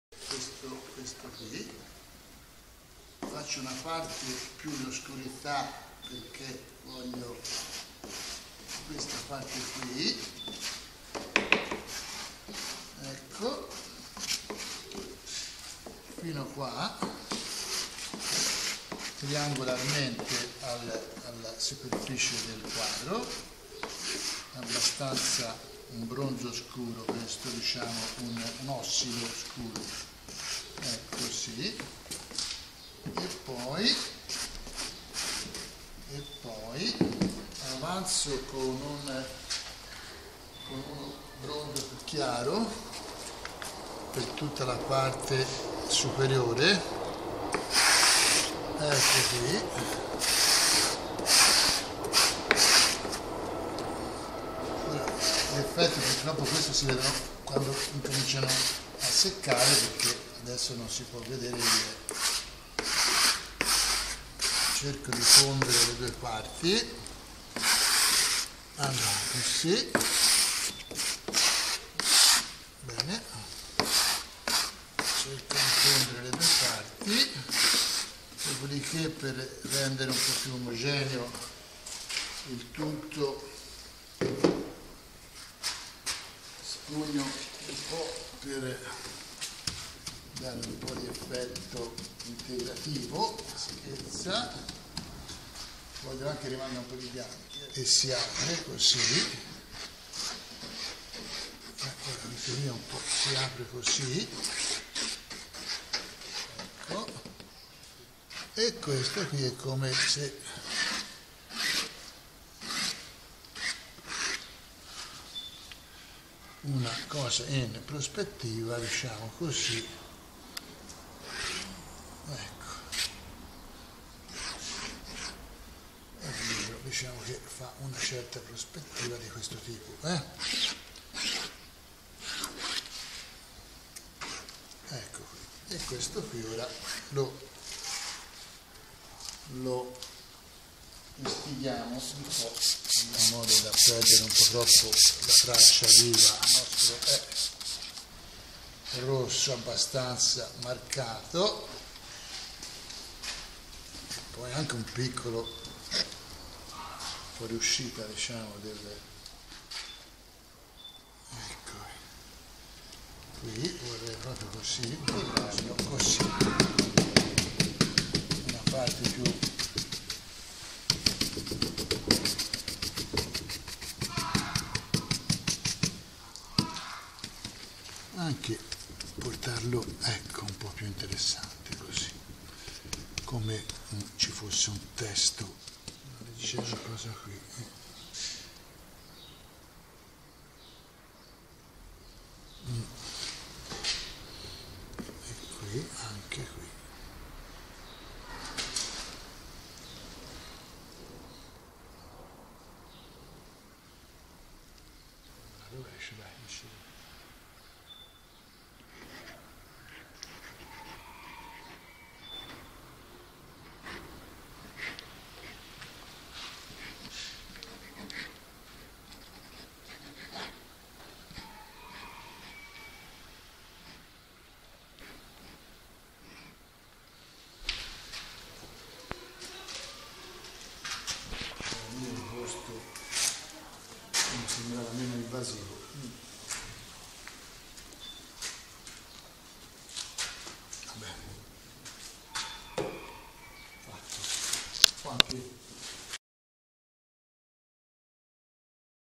"DIF/FUSIONI"
Named “DIF/FUSIONE” (Diffusion)

Are living works, living in the objective sense of the word, because they are composed by the alchemy of the constant corrosion of the products, the works will change over time, giving rise to an ORGANIC change that will manifest itself in adapting to the climatic reactions of the site.
With this peculiarity, Bettino wants to confront the "metamorphosis ..." by giving the works the opportunity to ADAPT to the surrounding realities for a continuous EVOLUTION.